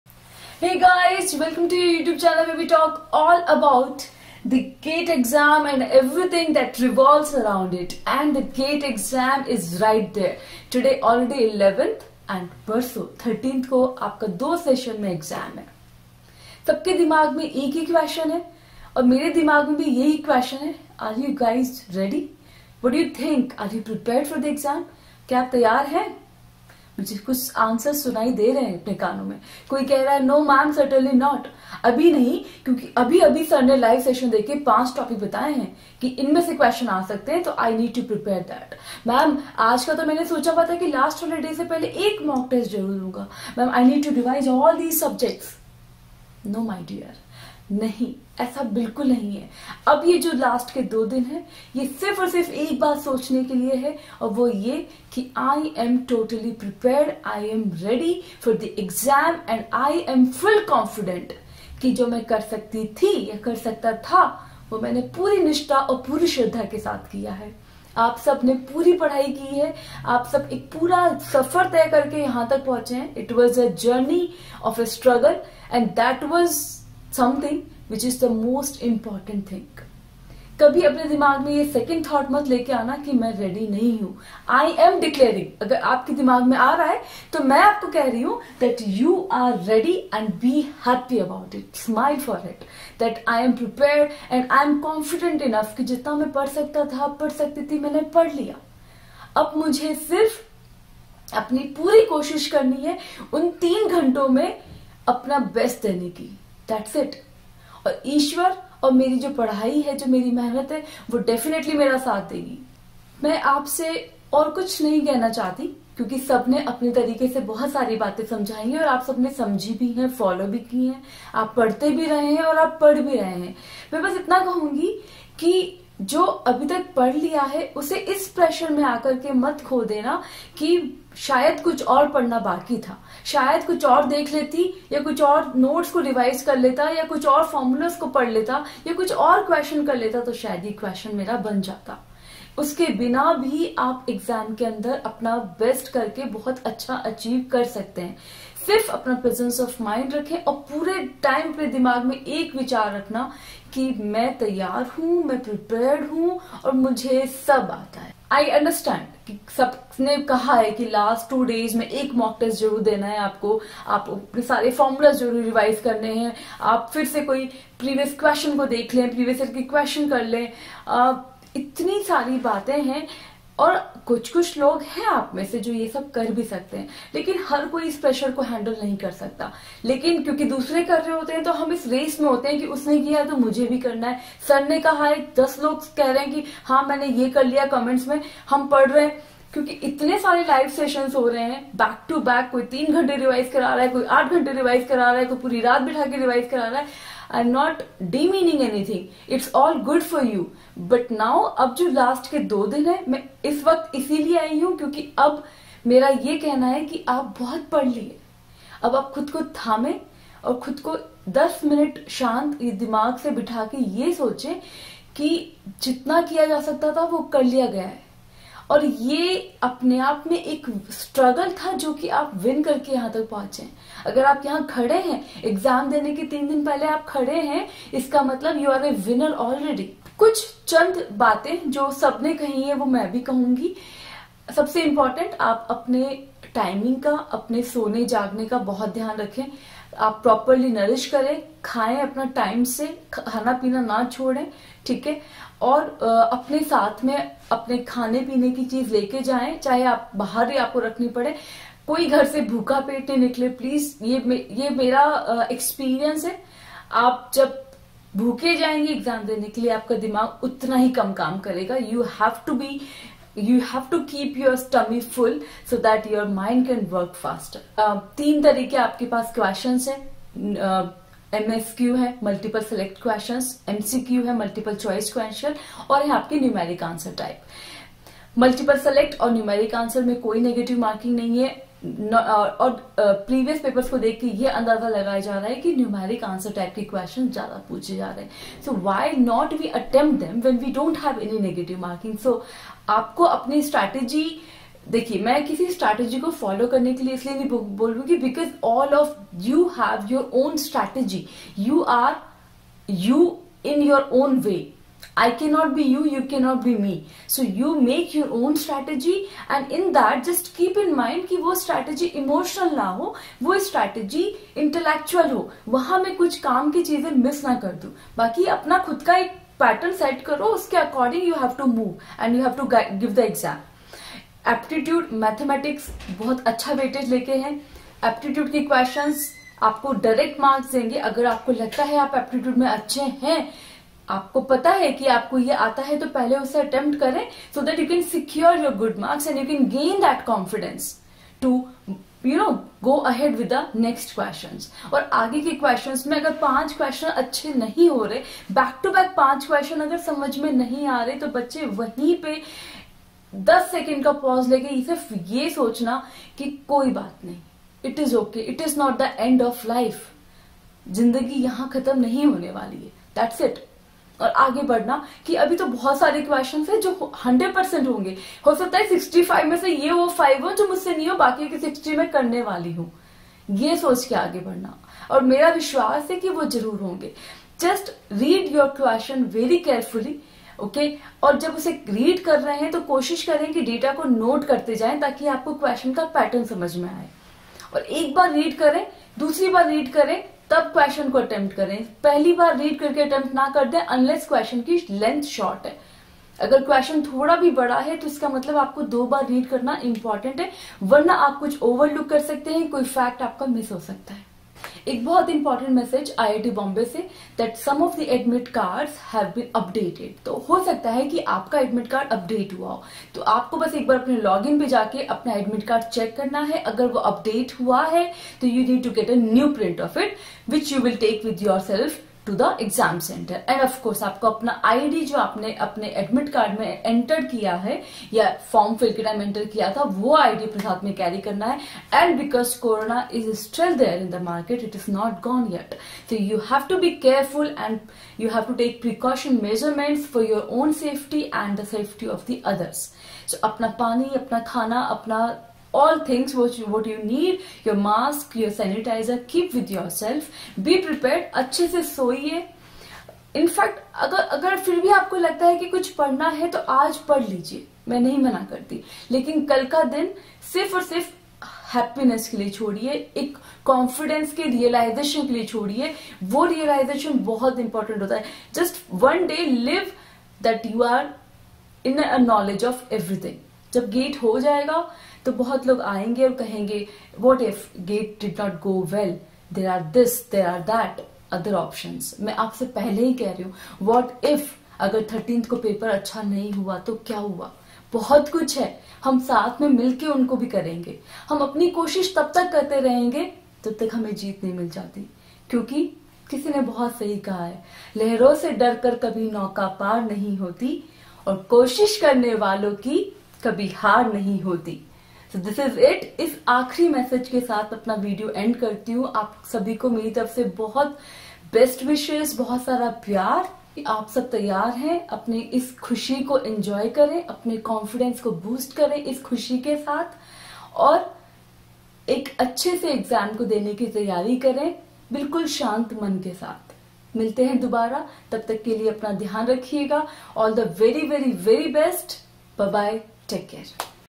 Are You READY For The GATE EXAM-2021? | Boost Your Confidence | Every GATE Aspirant Must Follow
Be Confident that you are prepared & you have done your best so far.